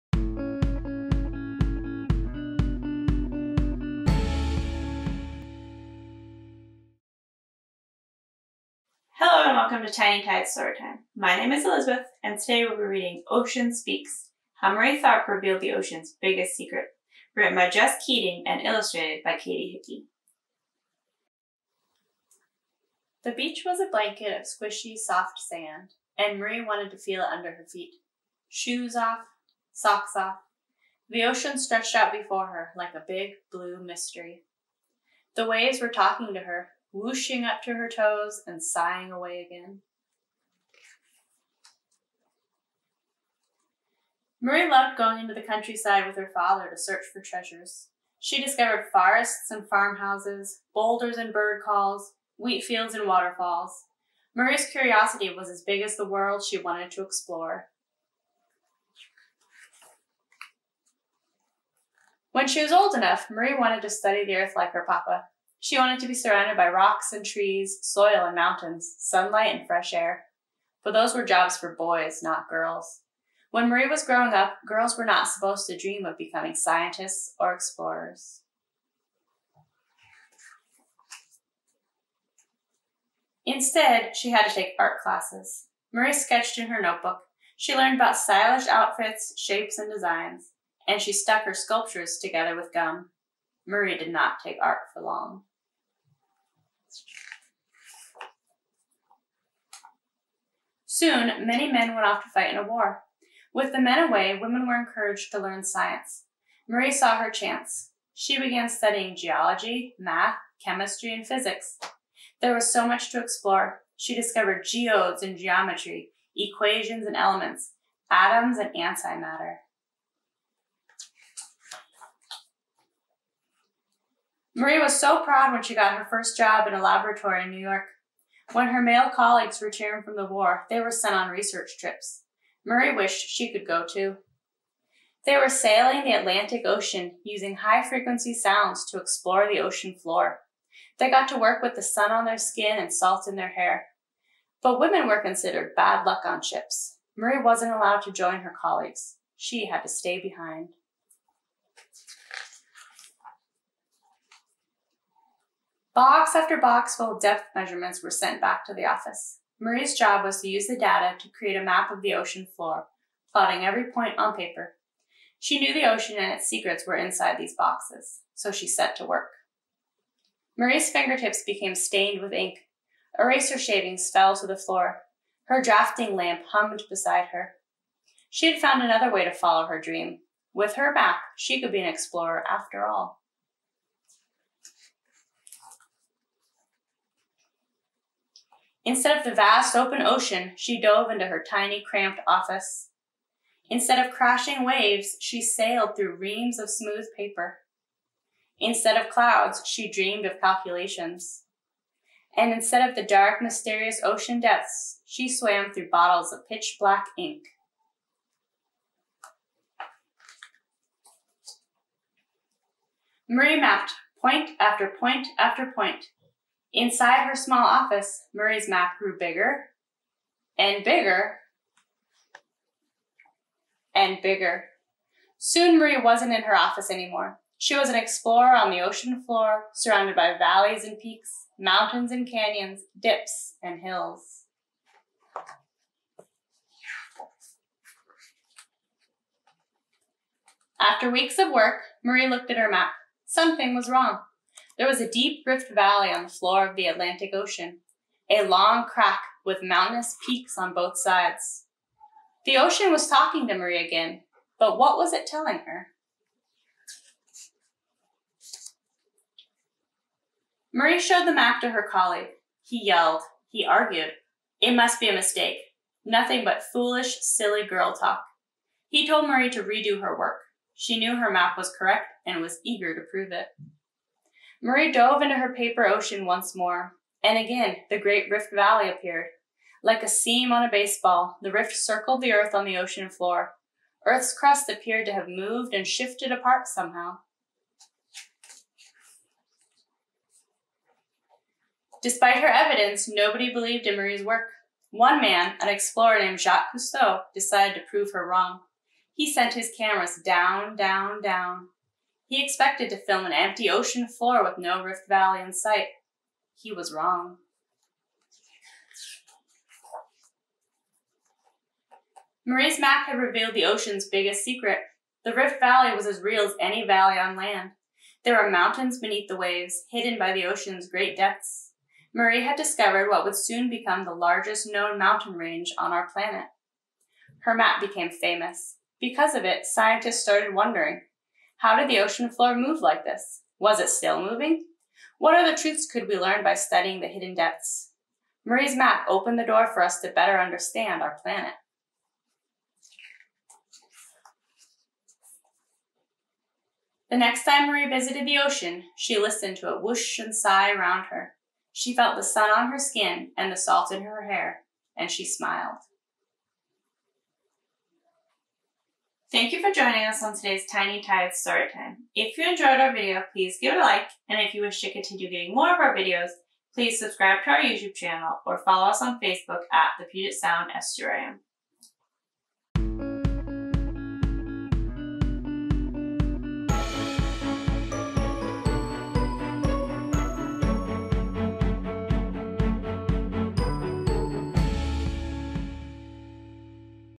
Tiny Tides: Ocean Speaks
Join us as our volunteer Elizabeth reads Ocean Speaks: How Marie Tharp Revealed the Ocean's Biggest Secret. Written by Jess Keating and illustrated by Katie Hickey.

To buy Ocean Speaks: How Marie Tharp Revealed the Ocean's Biggest Secret,

We do not have rights to Ocean Speaks: How Marie Tharp Revealed the Ocean's Biggest Secret.